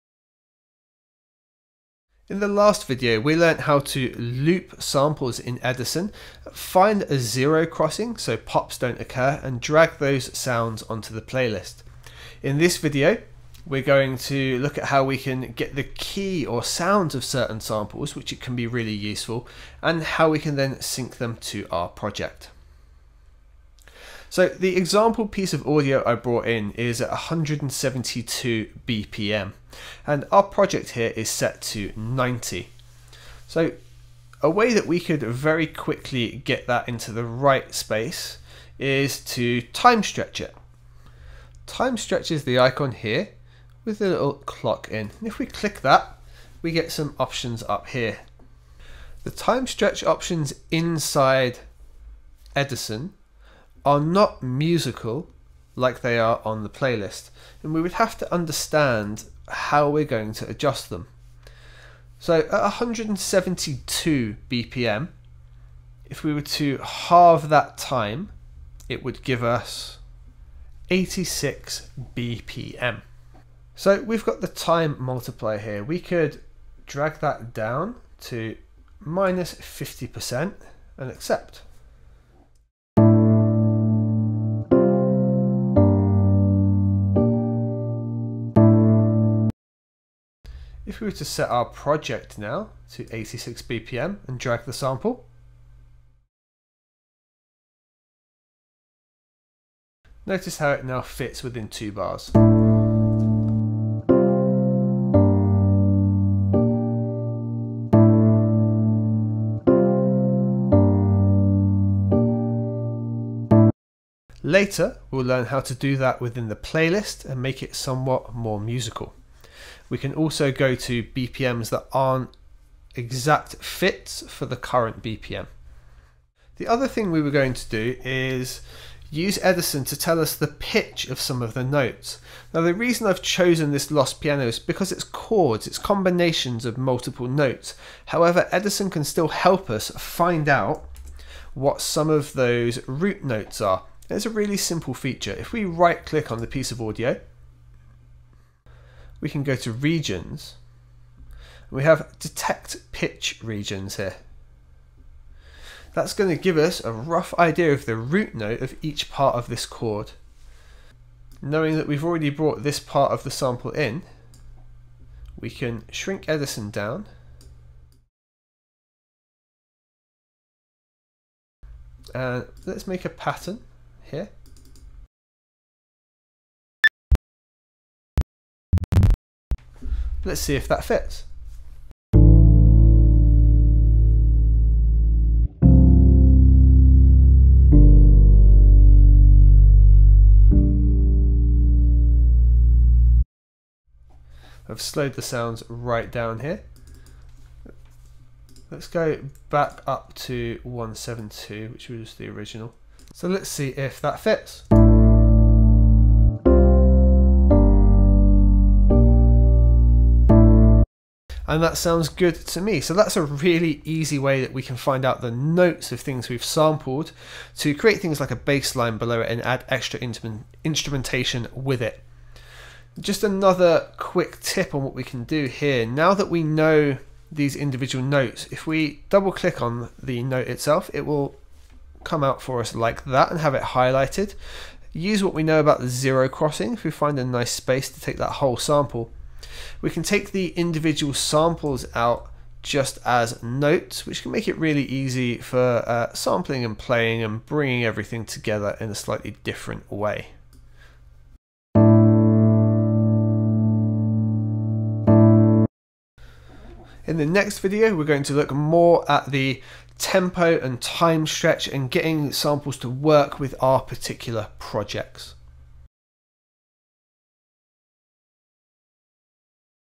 Find The Key of Sample in FL Studio When Sampling with FL Studio 20 #2
Sampling with FL Studio 20
In This video we learn how to Find The Key of Sample in FL Studio

🚀 For as little as $1 to get access to perks like tutorial files, projects & more

FREE STUFF